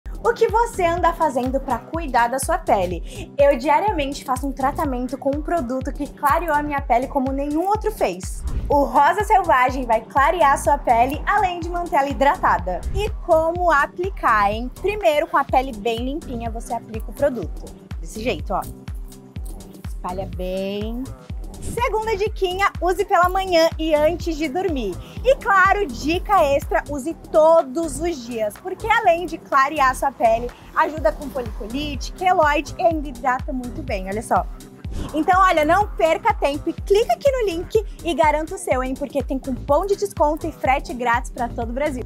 Cinthia Cruz usa Rosa Selvagem
Manchas na virilha e axilas são um pesadelo pra qualquer mulher! Tô certa?! E a terrível foliculite? nem se fala!

Pra acabar com esse pesadelo, vou te apresentar as melhores dicas para clarear manchas, acabar com a foliculite e irritação.

Tenho certeza que, se você colocar todas em prática, em menos de um mês, sua pele já vai estar completamente diferente!